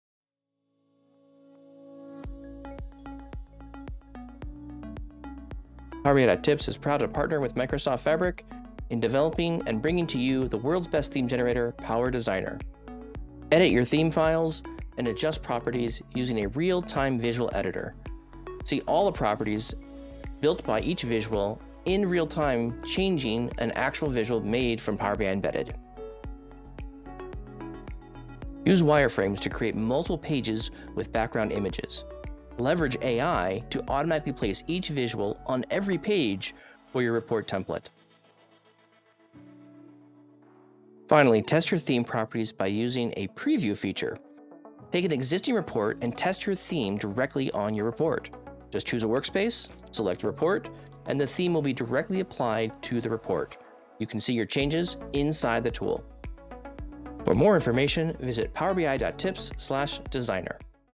Introducing Power Designer: Unleash Your Inner Report Wizard!
Hey there, data enthusiasts and report rockstars! Buckle up because we’ve got some electrifying news that’s about to turbocharge your reporting game. PowerBI.tips LLC, in a collaboration with Microsoft Fabric, is thrilled to announce that Power Designer is now Generally Available and officially out of Preview! Cue the confetti cannons — this is a real time saving application you won’t want to miss!

Say goodbye to bland, cookie-cutter reports and hello to a world of dazzling, highly stylized masterpieces. Power Designer is here to empower report authors like you to craft stunning, professional-grade reports and templates right within your Fabric Workspaces. Whether you’re a Power BI newbie or a seasoned pro, this tool is your new creative sidekick, ready to make your data pop like never before.

Try Power Designer today with a free 30 day trial